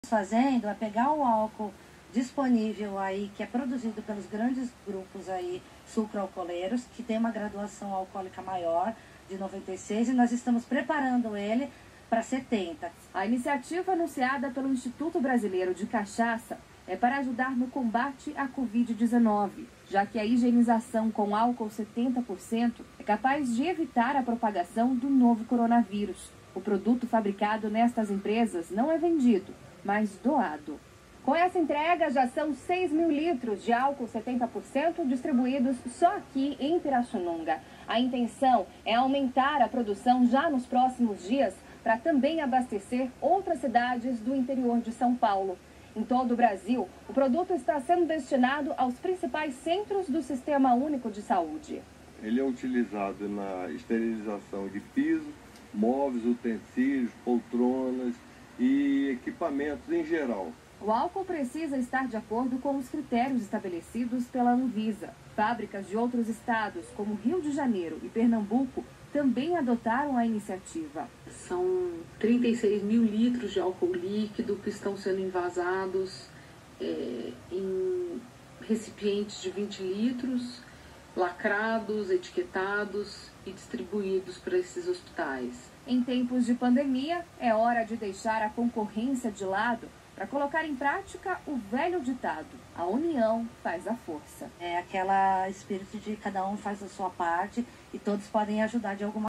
ÓTIMAS NOTÍCIAS FÁBRICAS DE CACHAÇAS DO BRASIL ESTÃO PRODUZINDO ÁLCOOL EM GEL PARÁ
PARÁ HOSPITAIS ESCOLAS ENTIDADES QUÊ CUIDA DE MORADORES DE RUA E AGILOS DE IDOSOS E CENTRO RECREATIVO PARÁ CRIANÇAS